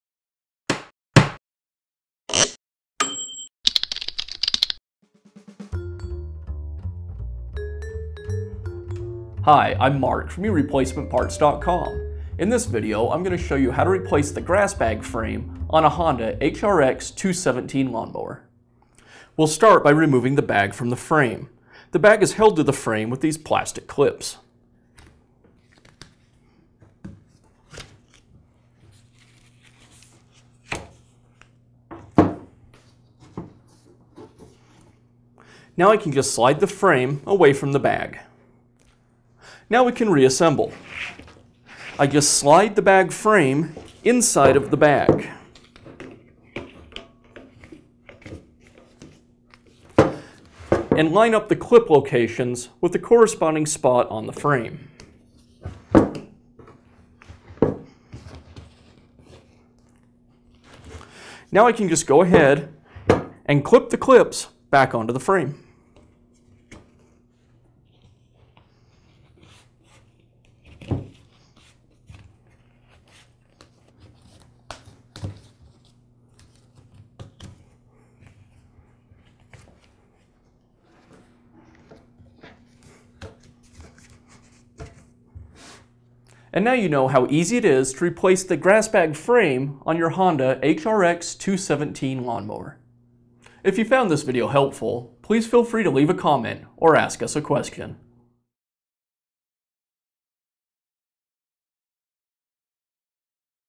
How to Replace the Grass Bag Frame on a Honda HRX217 Lawn Mower
Get a grass bag frame for your Honda HRX217 lawn mower

This tutorial will show you how to remove and install a grass bag frame on your Honda HRX217 lawn mower.